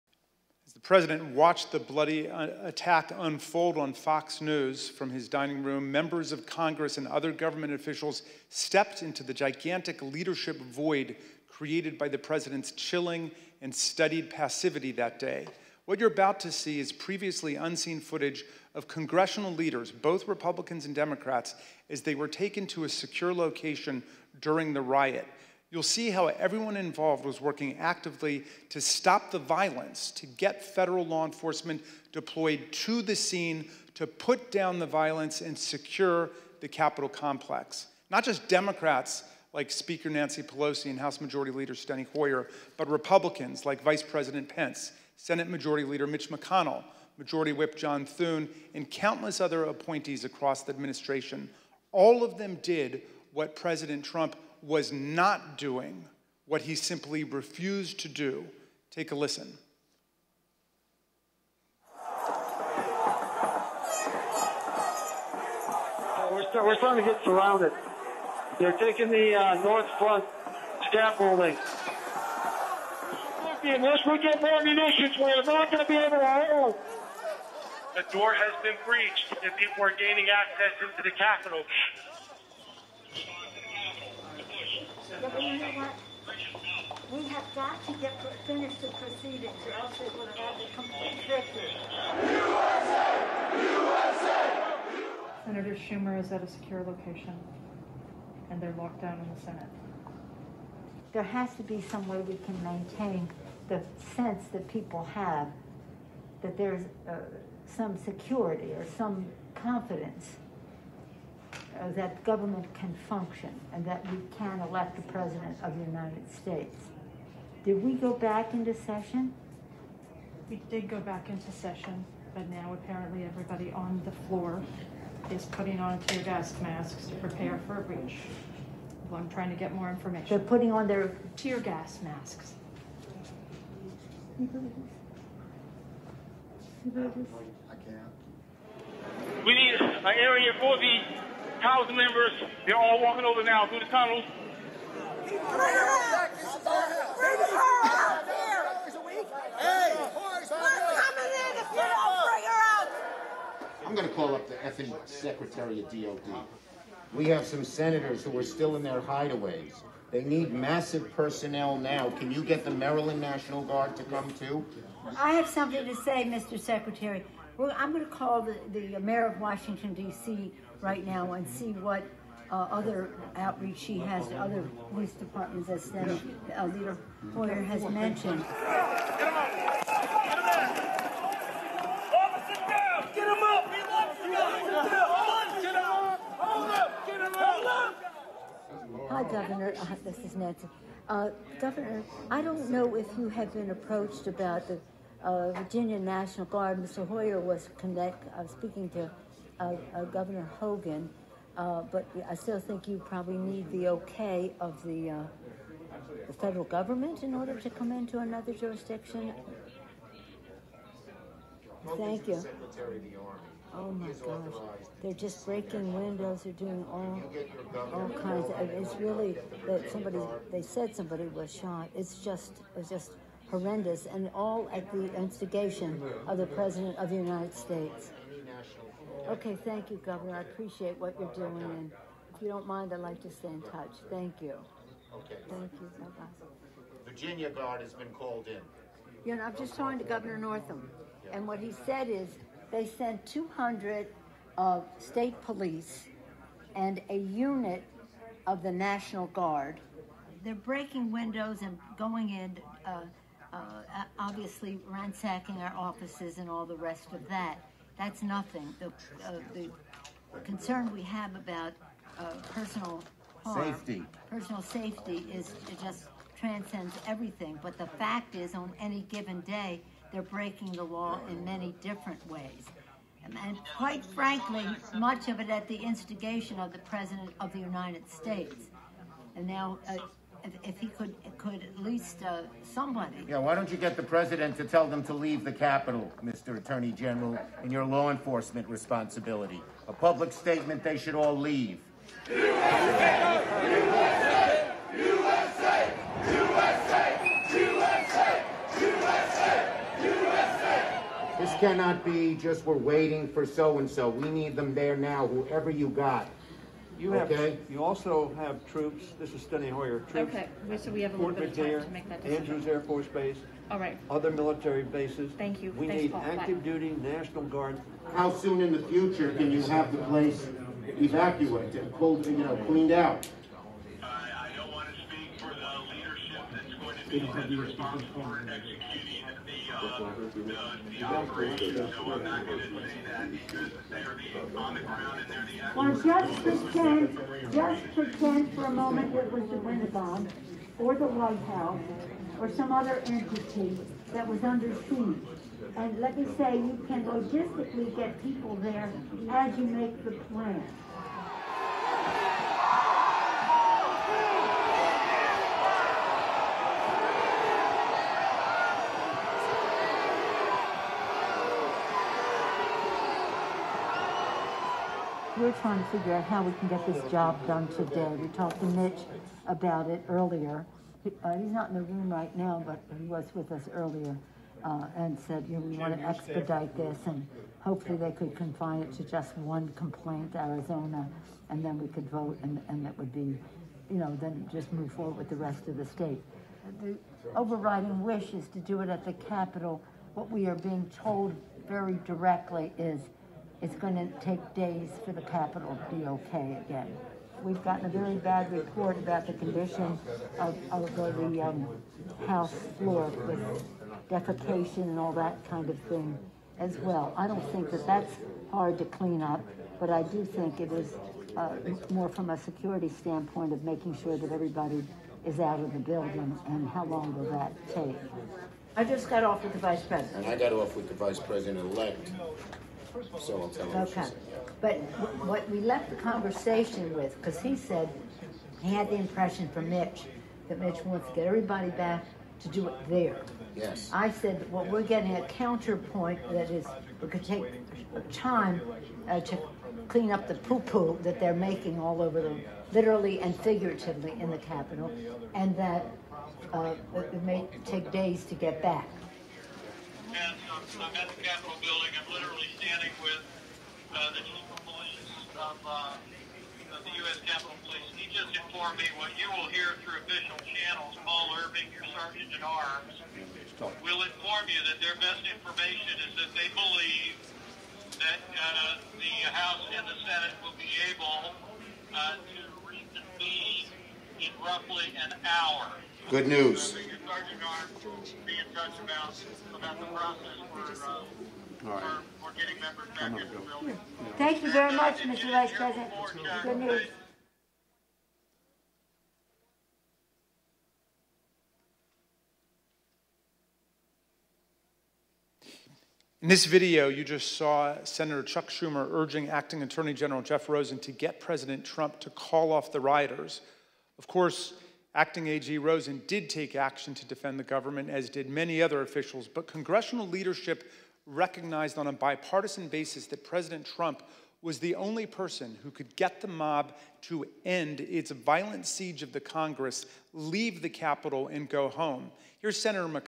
Congressional Leaders on January 6th
Rep. Jamie Raskin shows video of Congressional leaders on January 6th in secure locations calling for assistance at the Capitol.

Speaker Pelosi on January 6th: "Did we go back into session?"

Staff: "Now apparently everybody on the floor is putting on tear gas masks to prepare for a breach."

Speaker Pelosi: "They're putting on?

Staff: "Tear gas masks."

Pelosi: "Can you believe this?"